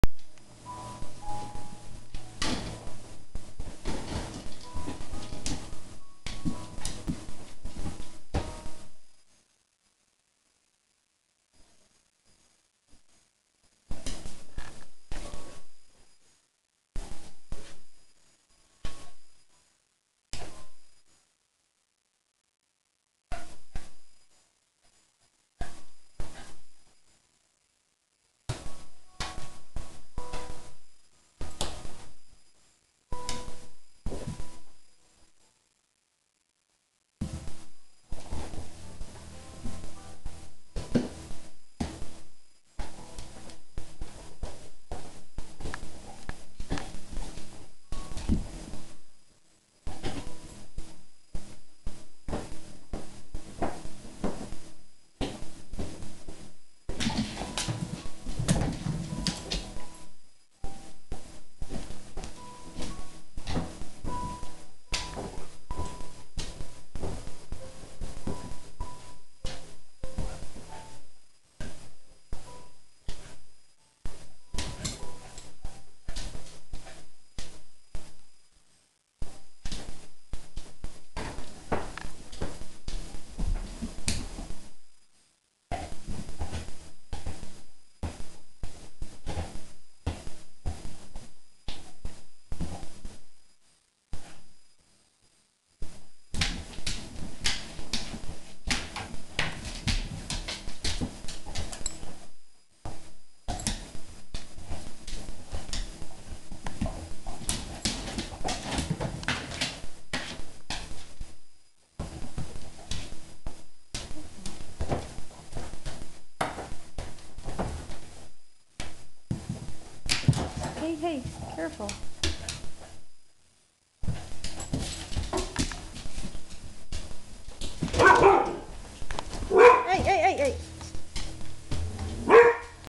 Reiker with a ball
I put Archie in his crate with a bone and reiker was playing with some toys in the bedroom